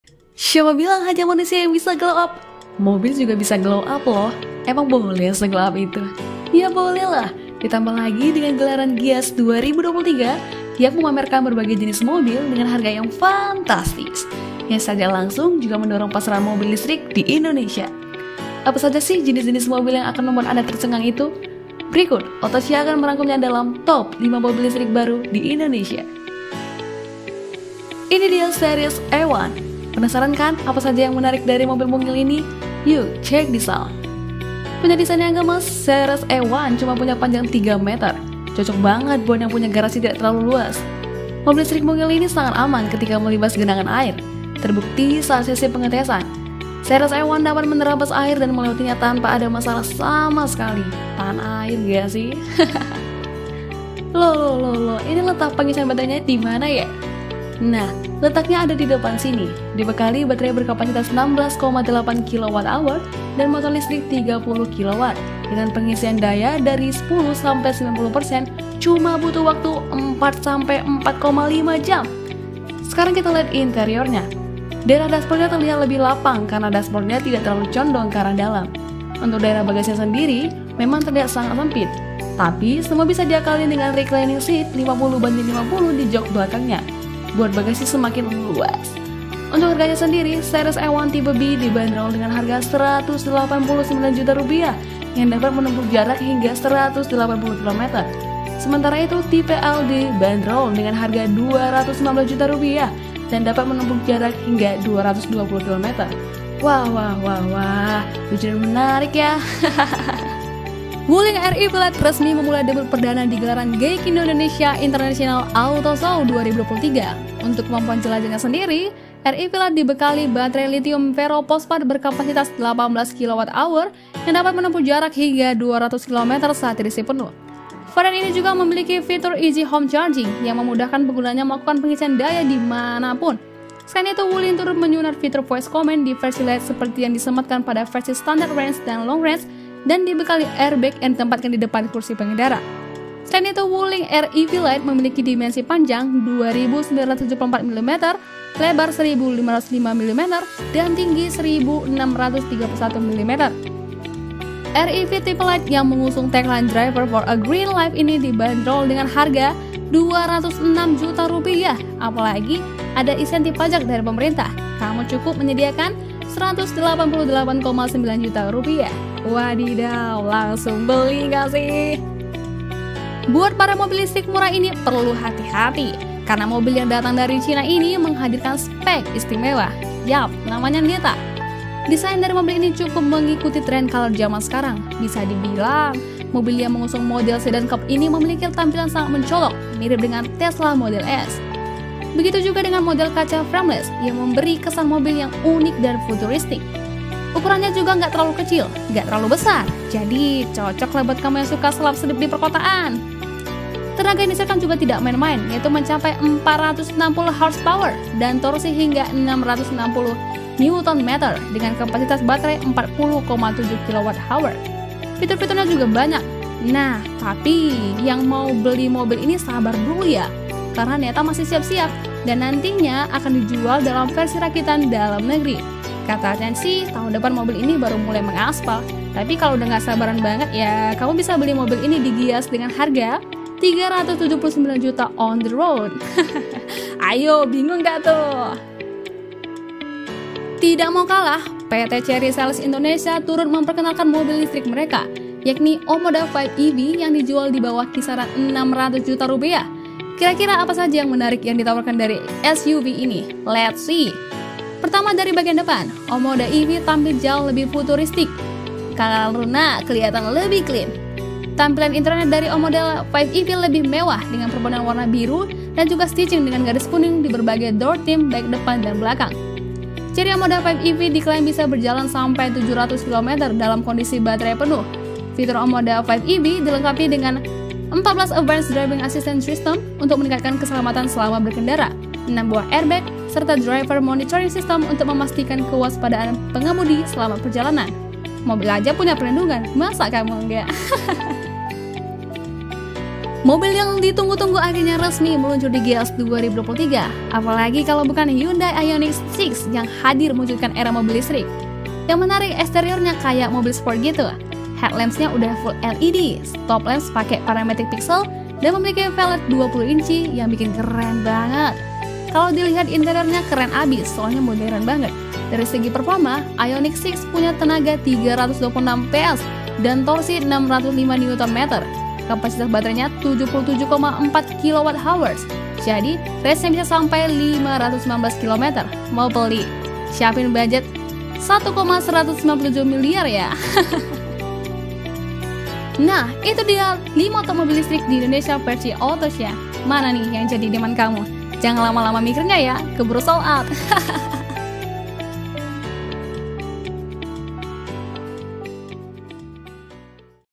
Top 5 Mobil Listrik yang Meluncur Di Indonesia | Ayo Beralih ke Mobil Listrik!
OTOSIA.COM -

Semua mobil listrik ini memberikan alternatif yang ramah lingkungan dan inovatif untuk mobilitas di Indonesia, dengan berbagai fitur dan keunggulan yang unik bagi masing-masing model.